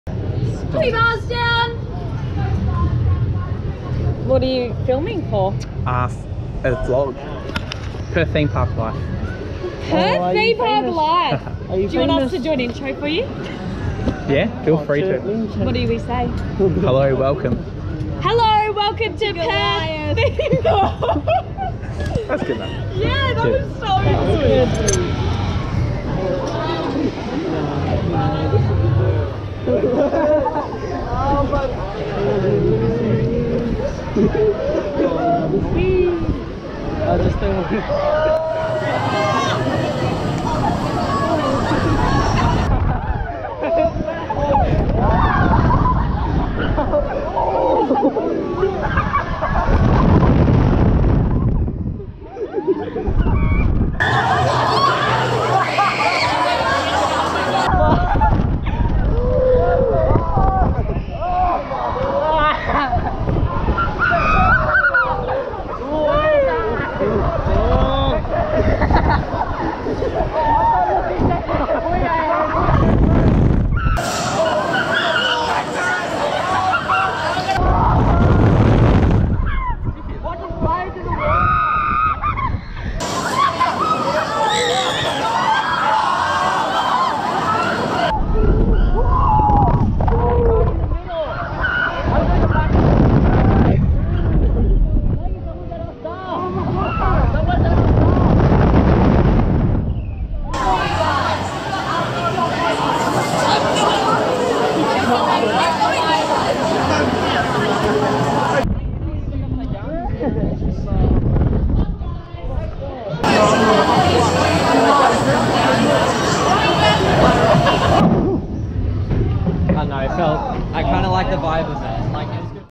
Goliath Evening POV
In the funniest intro ever, Goliath staff get chatting with Tim and welcome you to the vlog. Prepare yourself for an evening ride on Intamin Gyro Swing, Goliath.